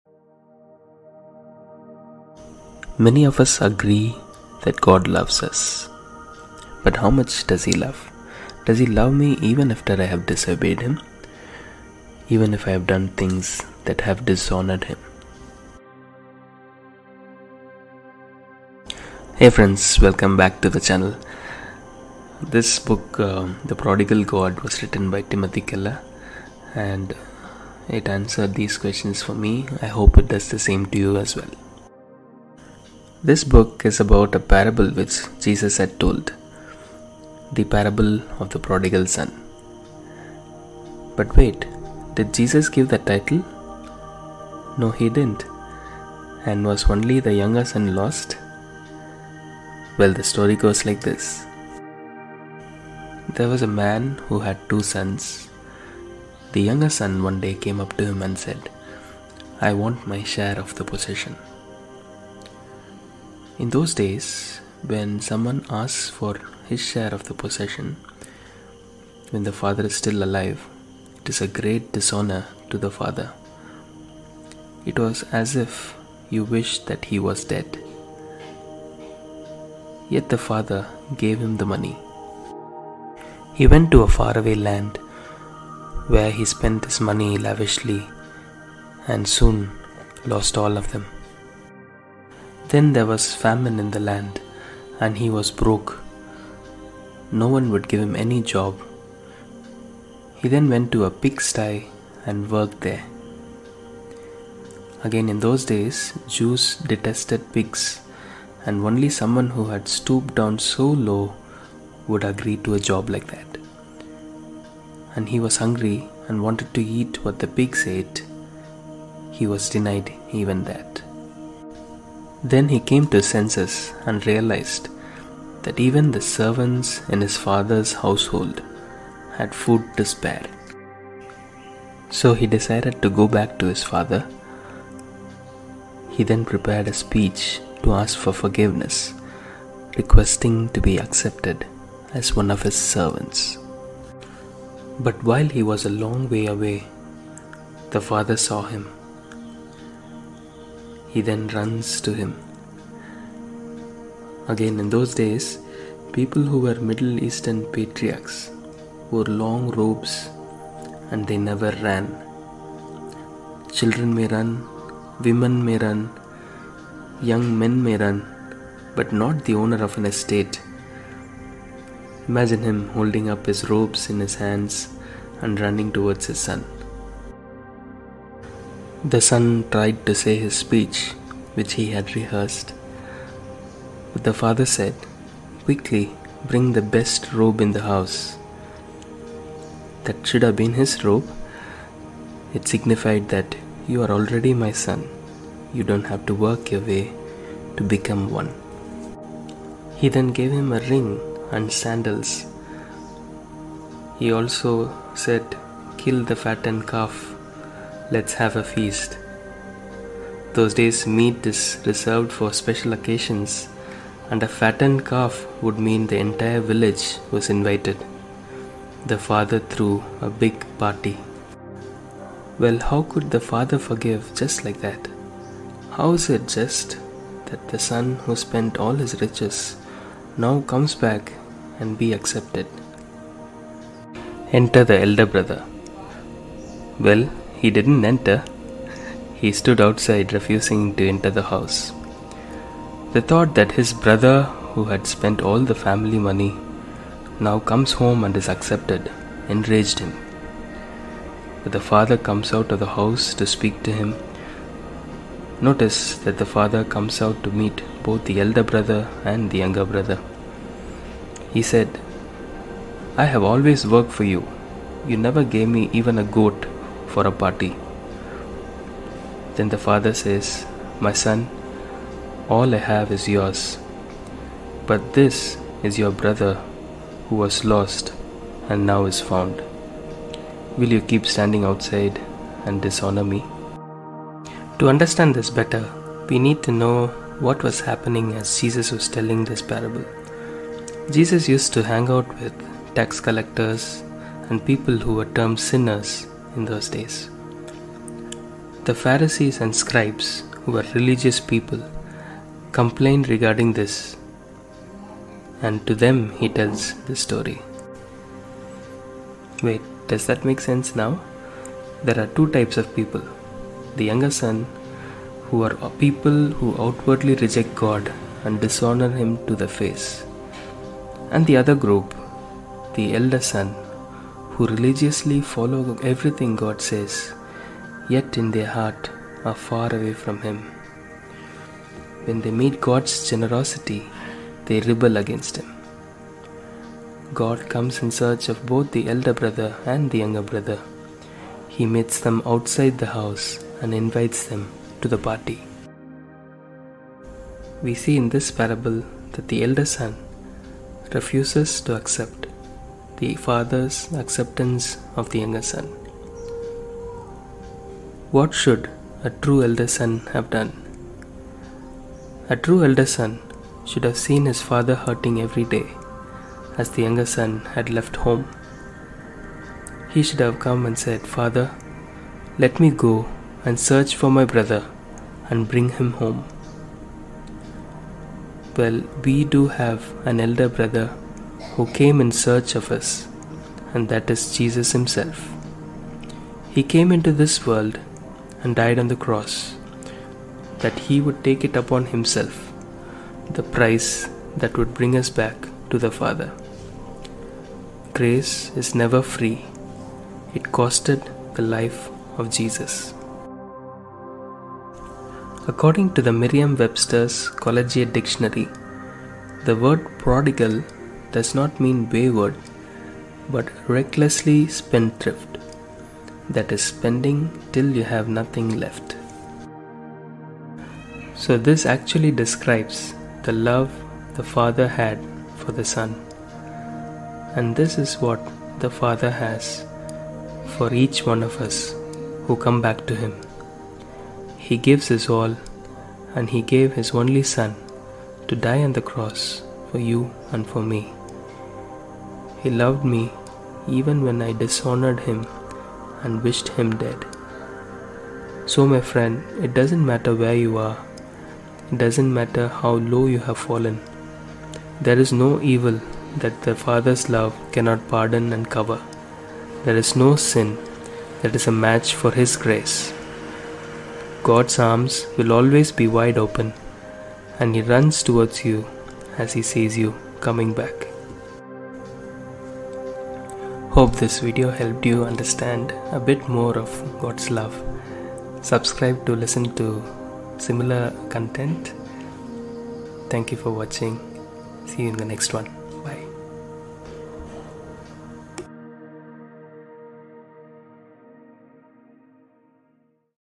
The book that helped me understand God’s love- The Prodigal God | Timothy Keller
This animation is a review of the book “The Prodigal God” by Timothy Keller. The link for the book is in the description below.